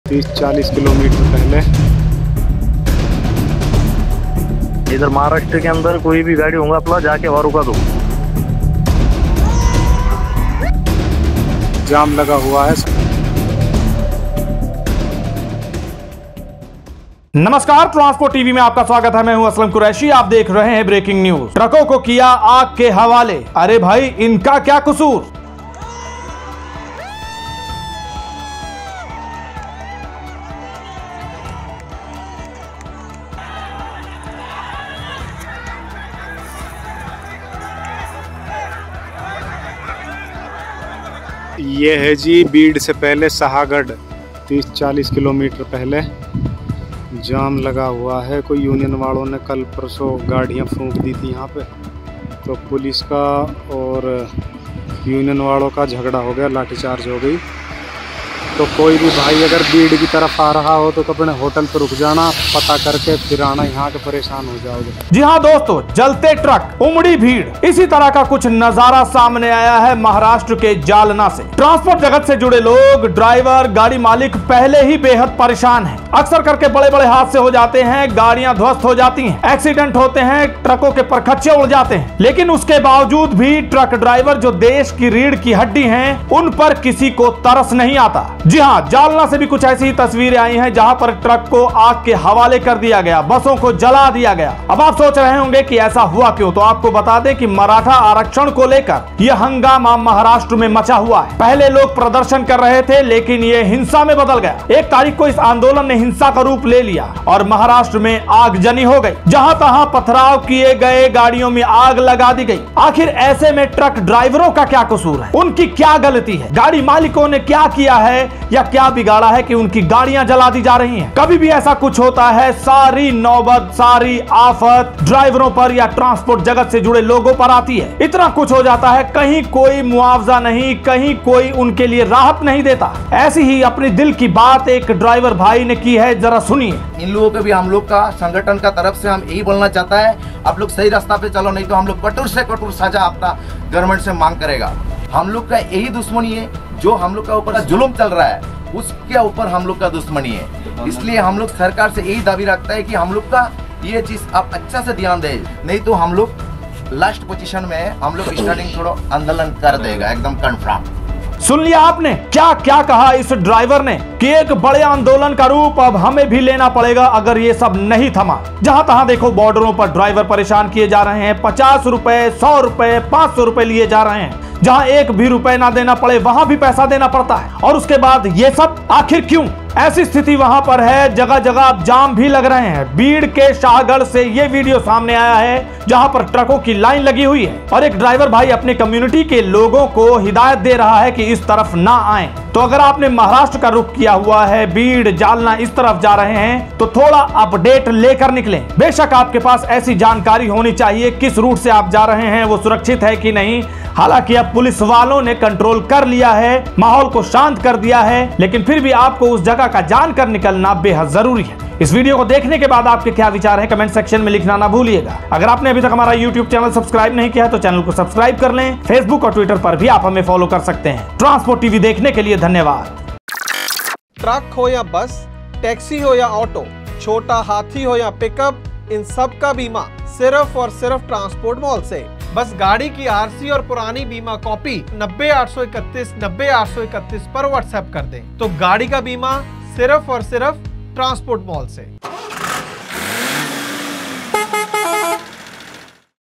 ट्रांसपोर्ट जगत को बनाया टारगेटइस राज्य में ट्रक बने आग के गोले | TRANSPORT TV | V-3492
यह मामला जालना महाराष्ट्र का बताया जा रहा है जहां मराठा आरक्षण को लेकर आंदोलन किया जा रहा है। जिसमें ट्रकों और बसों को आग लगा दी गई। सभी ड्राइवर भाईयों से निवेदन है इस तरफ जानकारी लेकर जाएं क्यूंकि आंदोलन के चलते रास्ते बंद हैं।